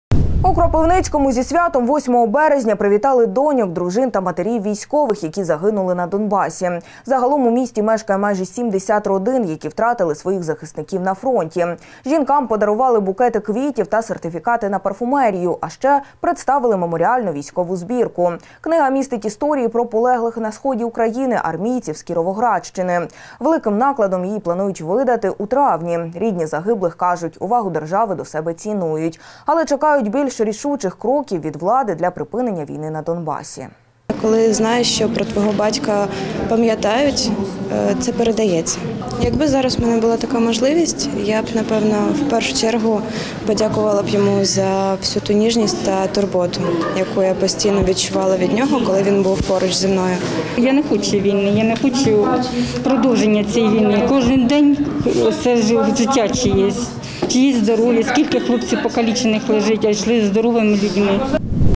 З нагоди 8 березня вітання отримали доньки, дружини та матері військових, які загинули в АТО/ООС
Берегині українських воїнів. У Кропивницькому з нагоди 8 березня вітання отримали доньки, дружини та матері військових, які загинули в АТО/ООС. Загалом у місті мешкає майже 70 родин, які втратили своїх захисників на фронті. Жінкам подарували букети квітів та сертифікати на парфумерію. А ще представили меморіальну військову збірку.